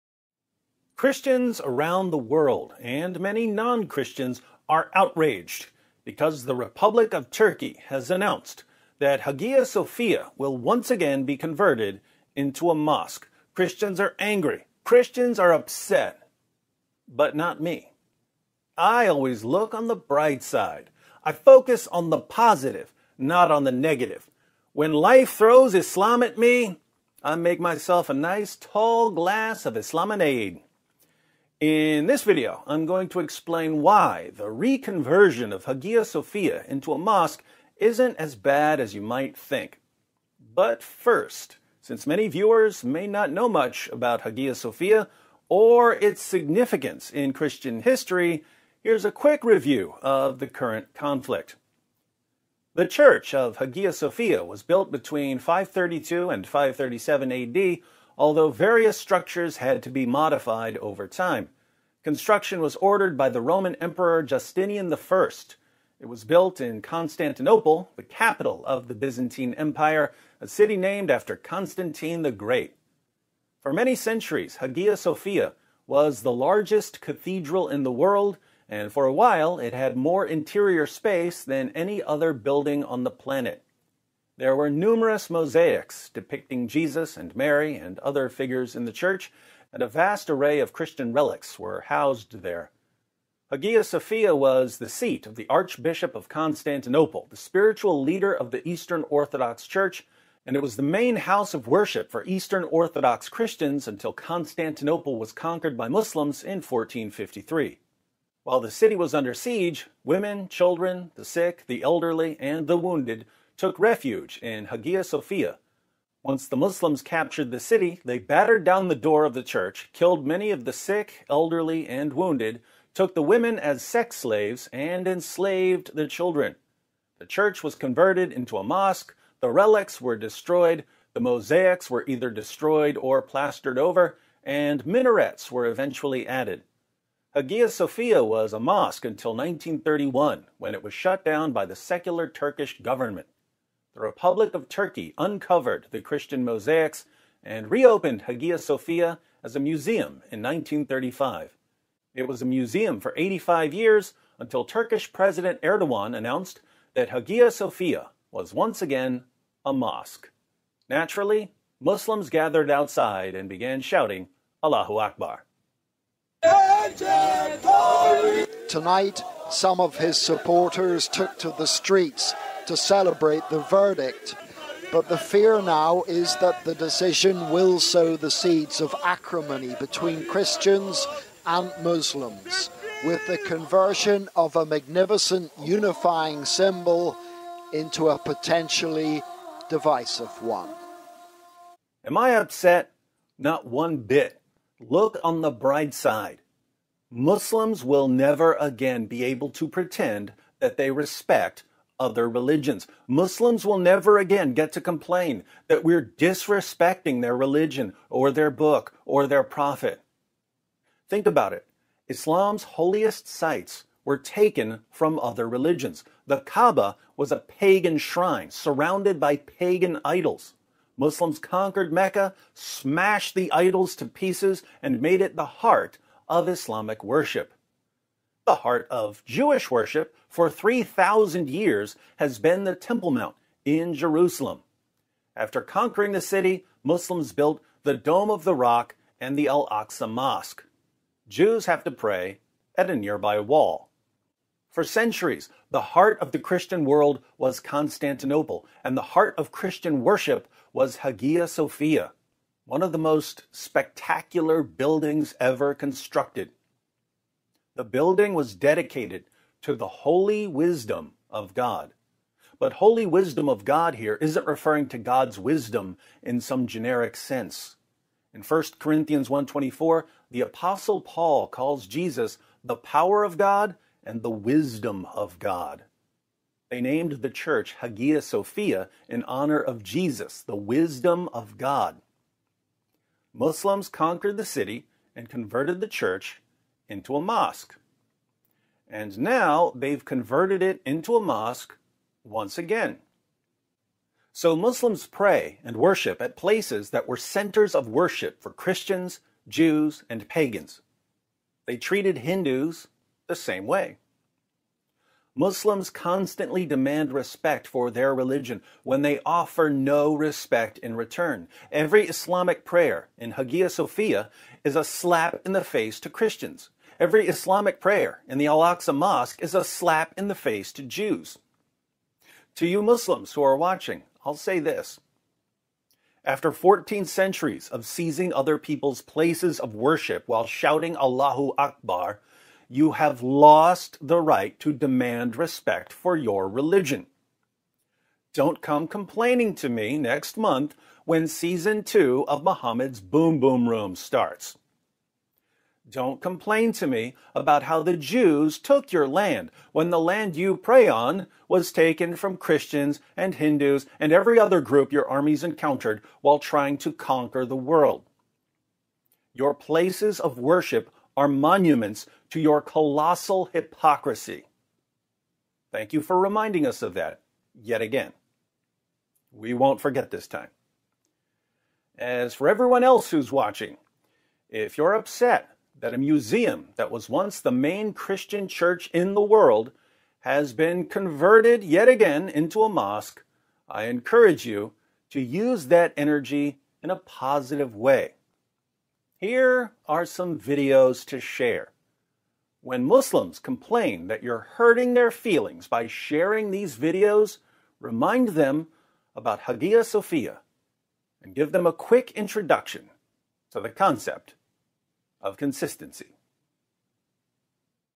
Sainte-Sophie redevient une mosquée (mais ce n'est pas aussi terrible que vous le pensez !)
Le 10 juillet 2020, le président turc Erdogan a annoncé que Sainte-Sophie ("Sainte Sagesse"), qui était un musée depuis 1935, était à nouveau convertie en mosquée. Depuis que l'église Sainte-Sophie a été construite par des chrétiens en l'honneur de Jésus-Christ, beaucoup sont bouleversés par sa reconversion en mosquée. Cependant, David Wood a une perspective différente.